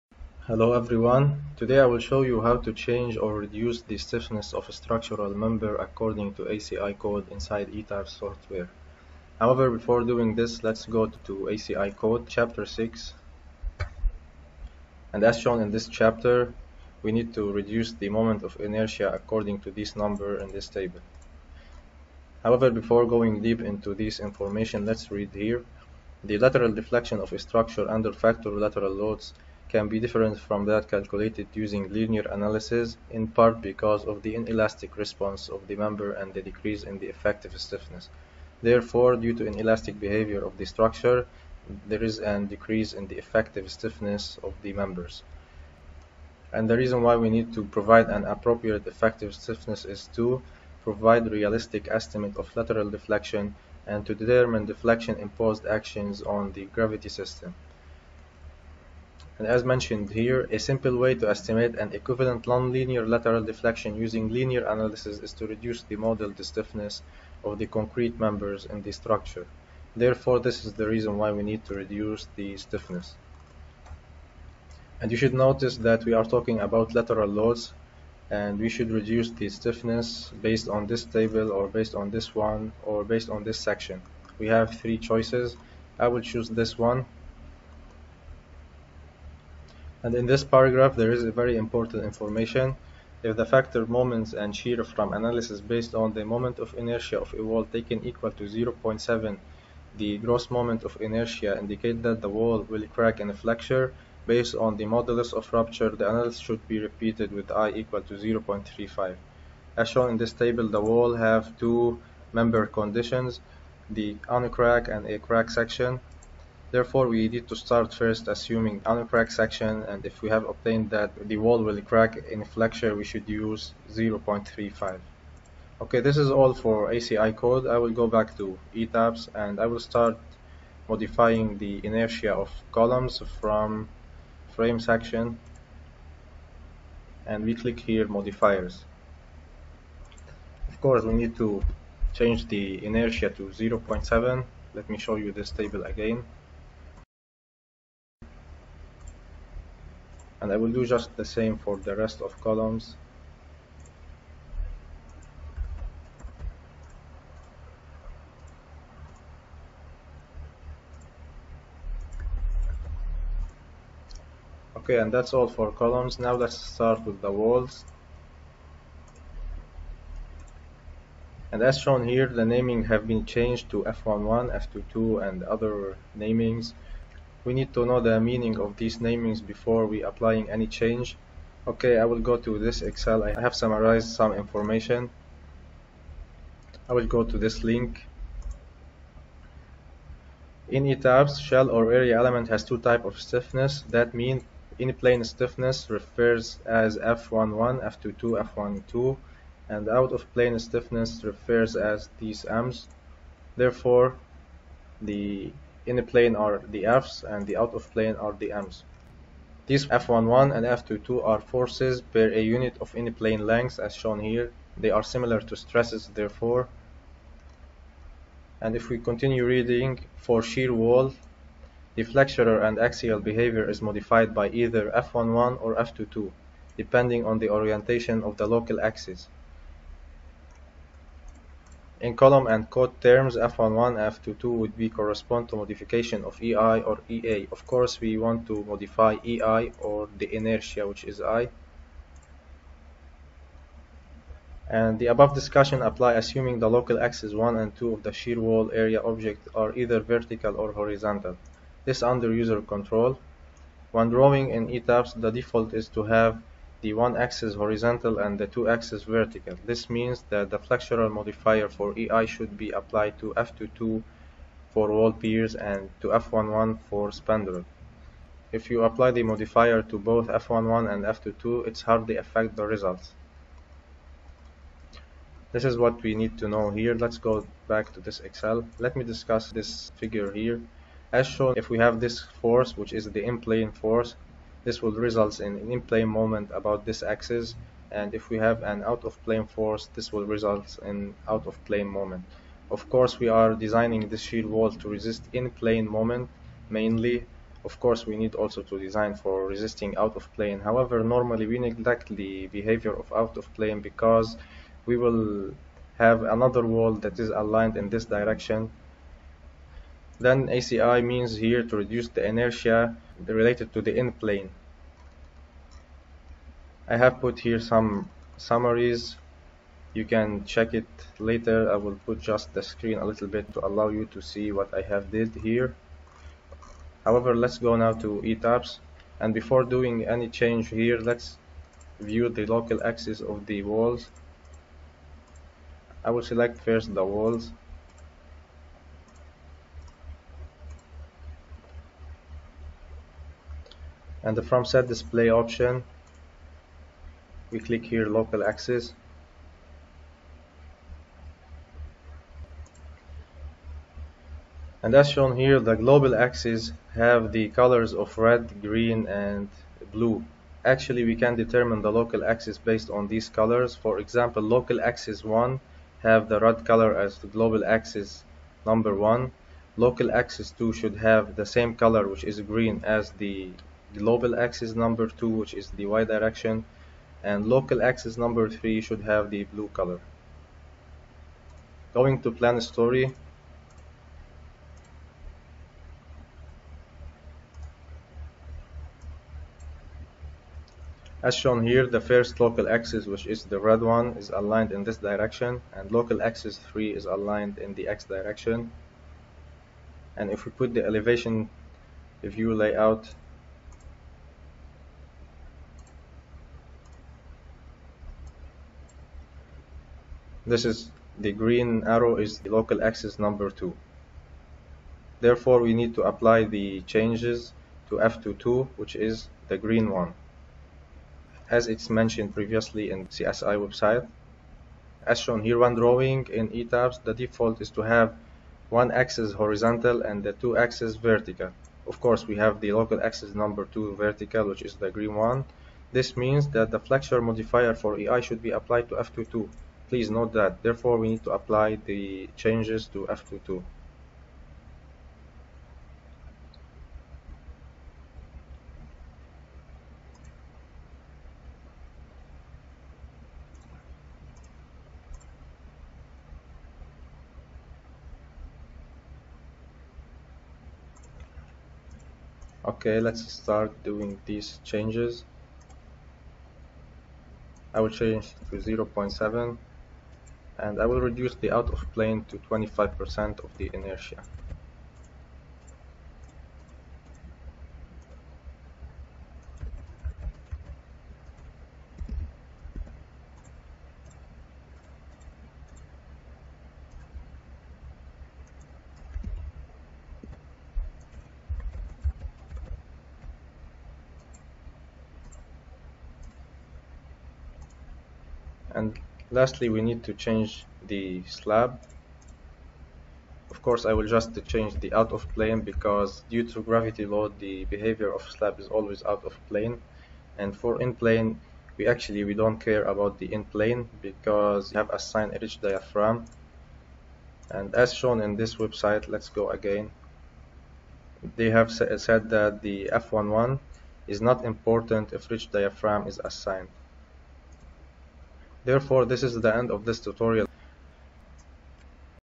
How to change the flexural stiffness of walls, column, and slab in ETABS software (Lec13)
This video shows how to change the flexural stiffness (moment of inertia) of members by modifying the stiffness modifiers in ETABS software.